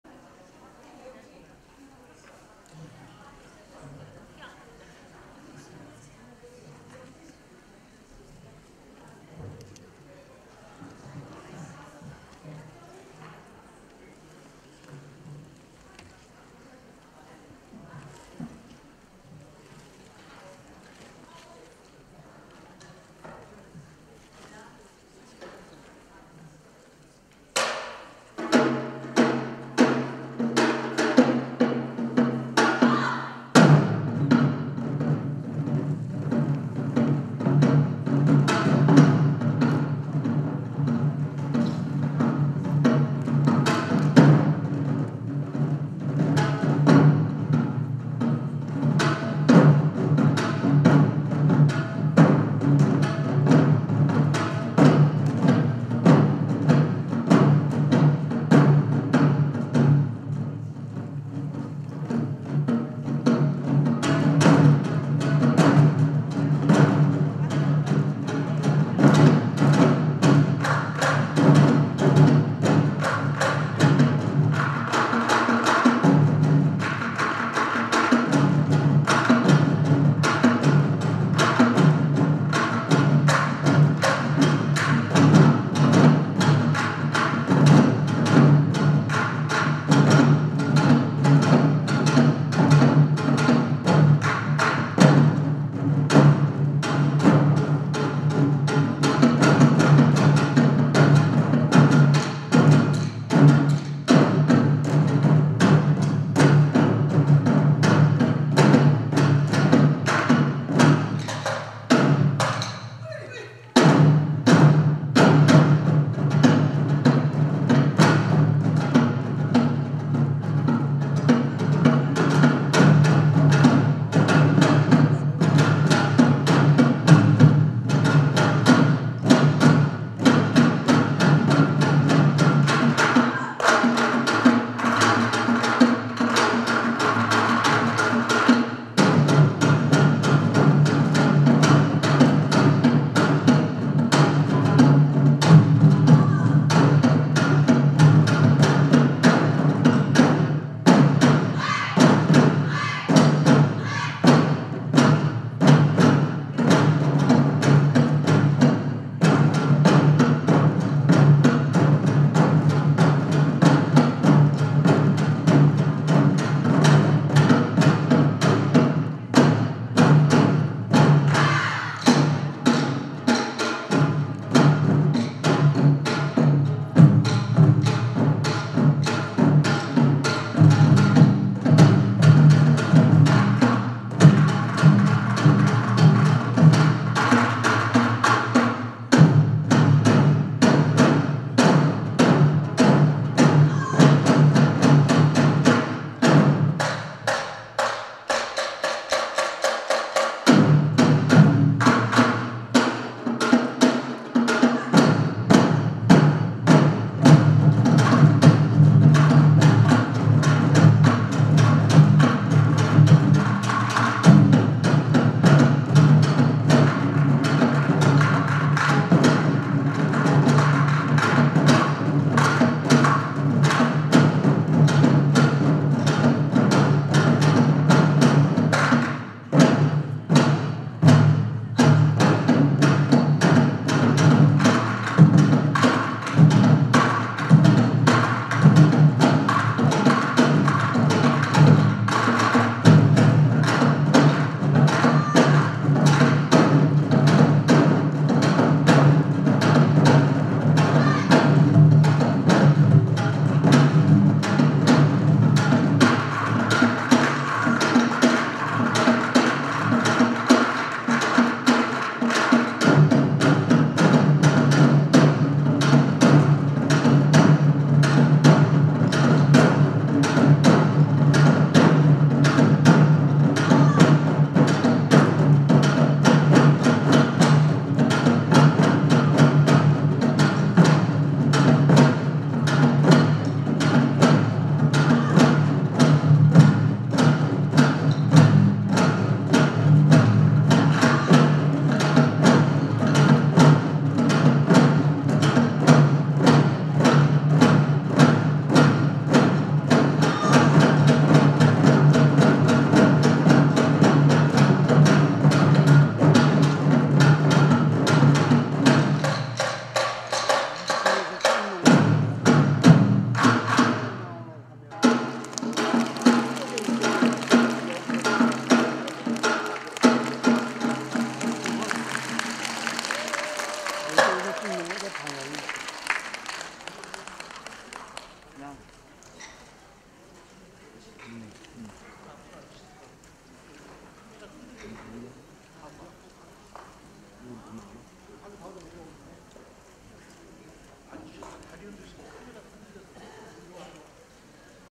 한타래무용단(삼복춤의향연)제10회동아시아문화예술축제인천송도글로벌캠퍼스19-10-26영상감독 장털보
문화예술경연대회
인천 송도 글로벌캠퍼스19-10-26